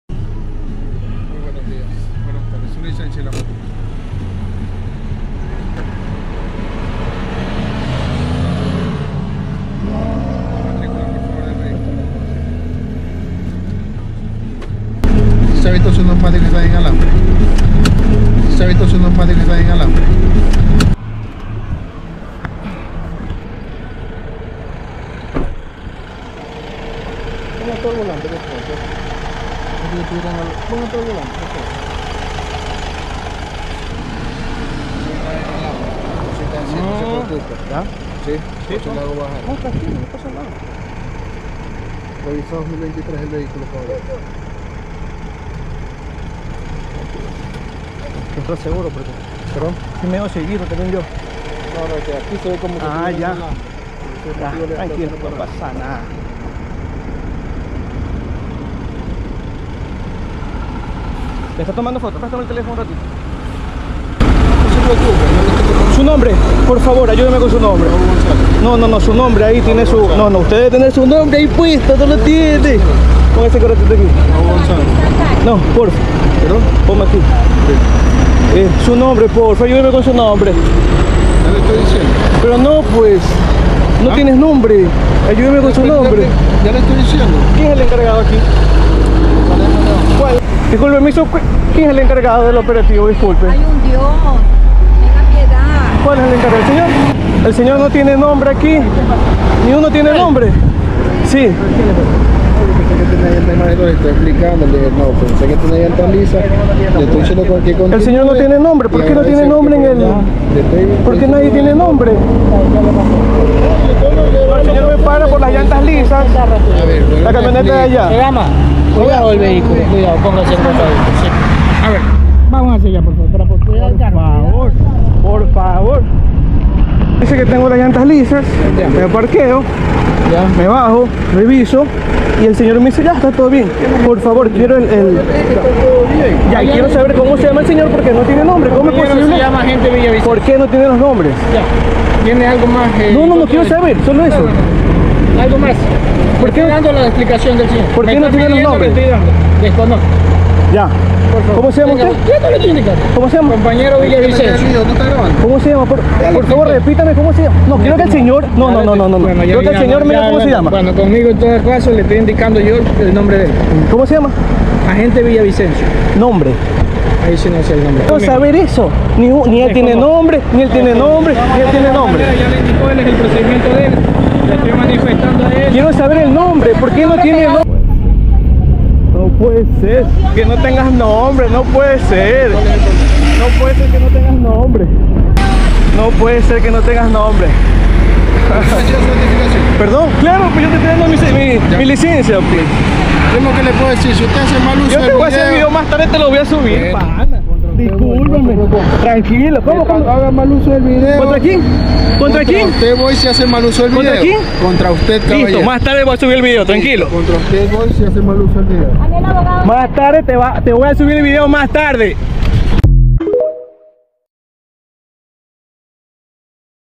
CTE separa a grupo de agentes de tránsito que armaron operativo sin identificación. Guayas
¿Por qué no tienen sus nombres? Esa fue la pregunta que le hizo un conductor a un grupo de agentes de tránsito luego de ser detenido en una revisión vehicular de rutina en vía a Costa, en Guayaquil.

El conductor grabó la escena. Allí se evidencia que el conductor fue detenido por transitar presuntamente con “llantas lisas”, sin embargo, el agente de tránsito no portaba su nombre en el uniforme de la institución, tal como estipula la ley.

La Comisión de Tránsito del Ecuador  se pronunció e indicó que «se tomó la medida de separar del Grupo de Control al Agente de Tránsito por infringir el reglamento y se procederá con la respectiva sanción».

“Esta administración reafirma su compromiso con la transparencia y no permitirá que se repitan estos hechos suscitados”, agrega el pronunciamiento de la CTE.

Fuente video: @juankip1104